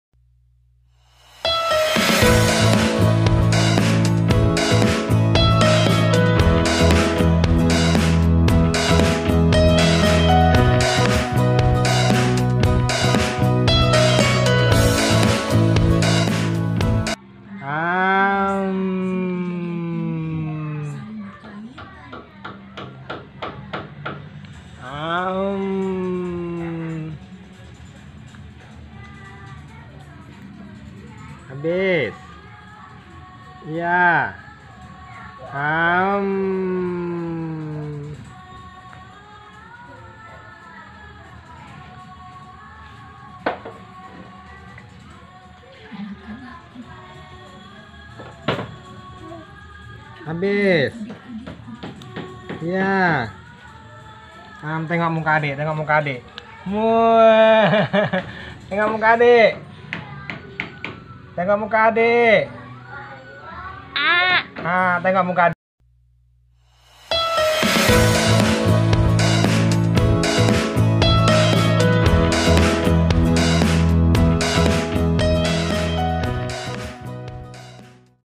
Baby comel doyan makan || wajah belepotan
Ekspresi lucu,comel si baby alea.
Saat makan jajanan.wajahnya belepotan coklat.baby comel doyan makan.